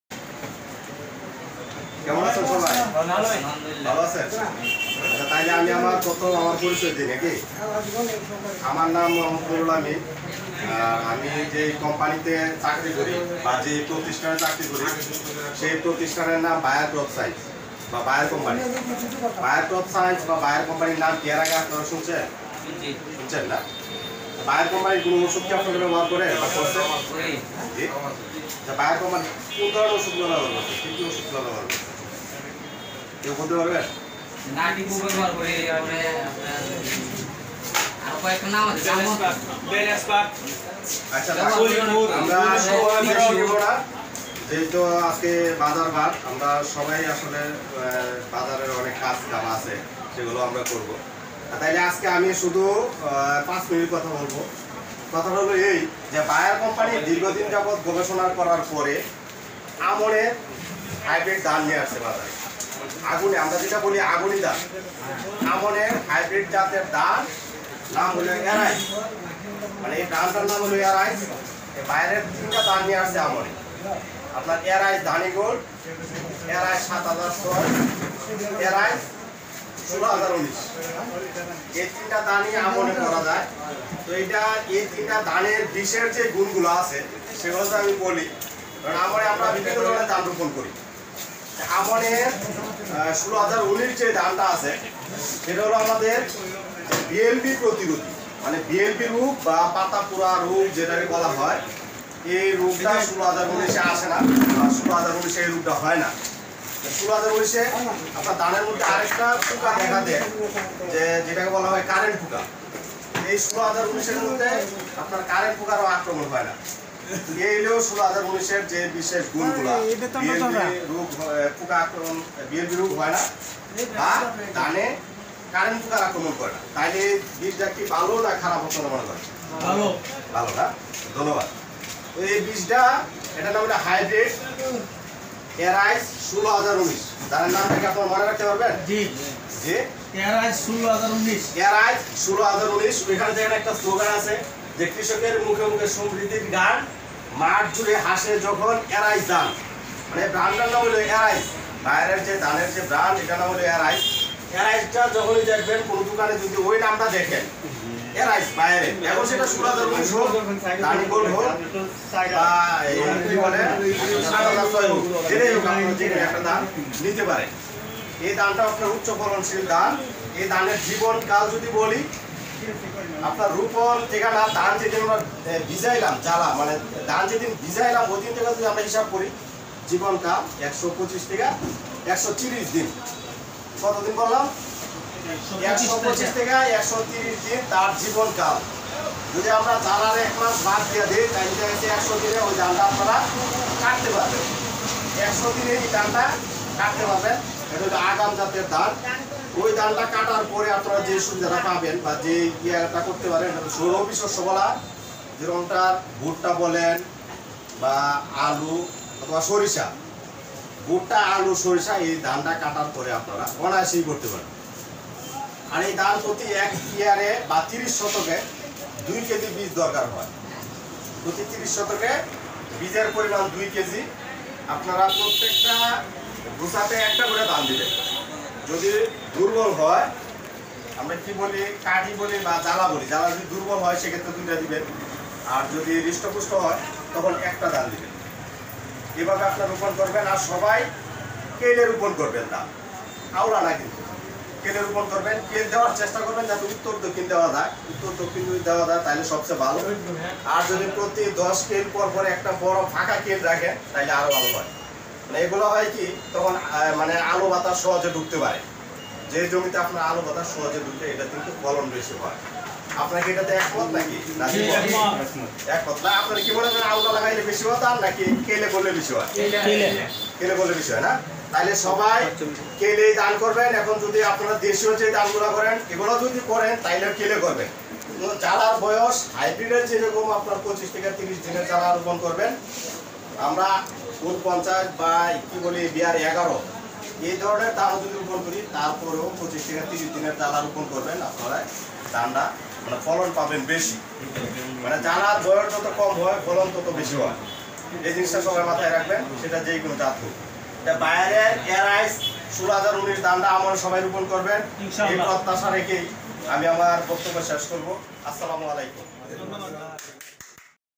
কৃষকের মুখে মুখে সমৃিদ্ধর গান, মাঠ জুড়ে হাসে যখন অ্যারাইজ ধান,হাইব্রিড ধানের জগতে, নতুন বিস্ময়,
অ্যারাইজ,আই এন এইচ১৬০১৯।